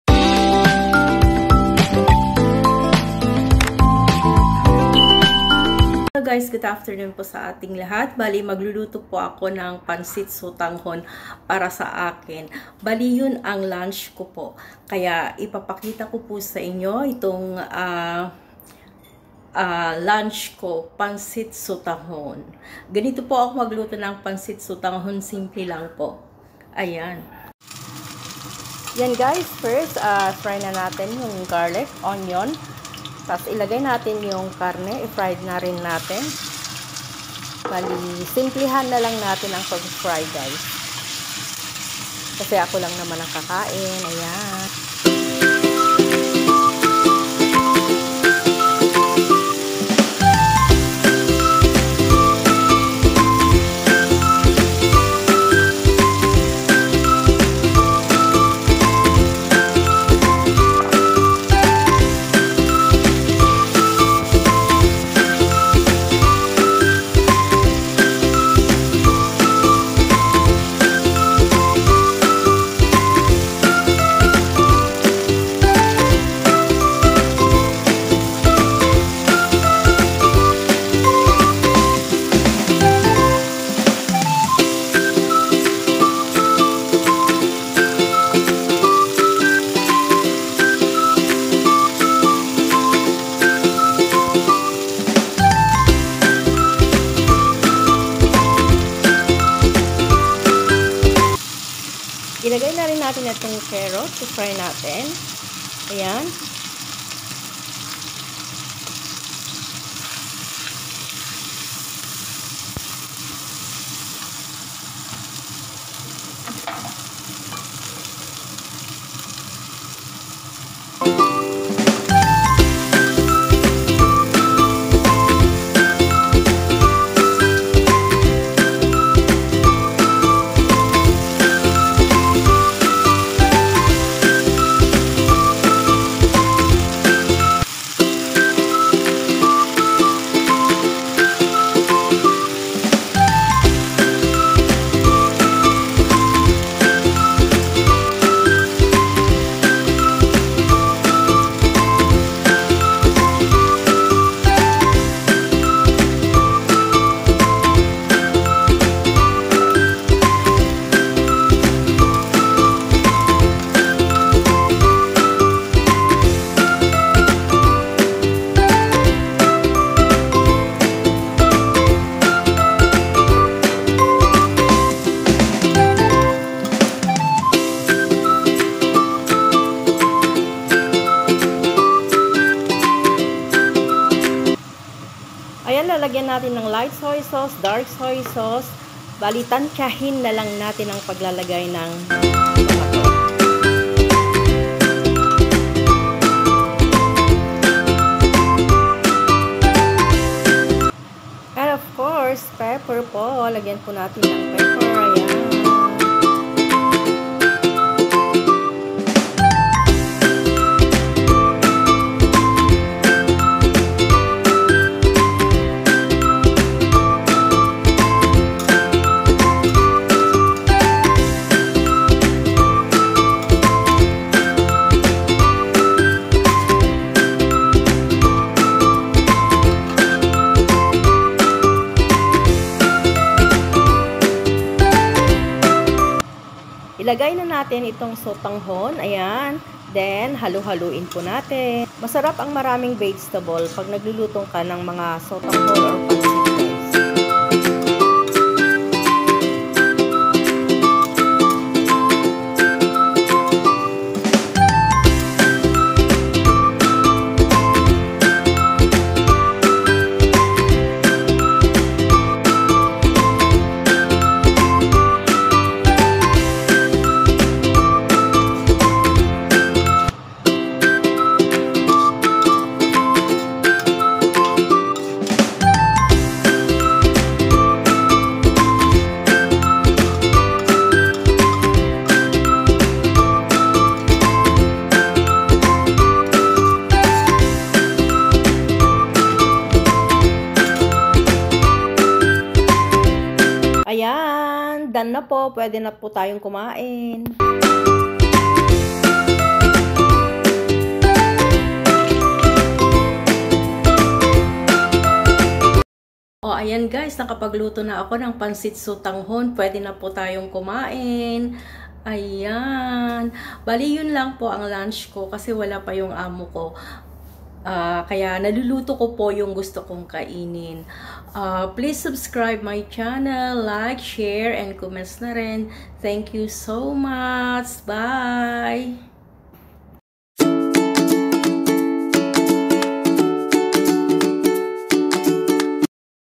Pansit Sotanghon /simple cooking@momsheetvblog
Cooking pansit sotanghon in a simple way for my lunch.Watch it guys.Thank.you!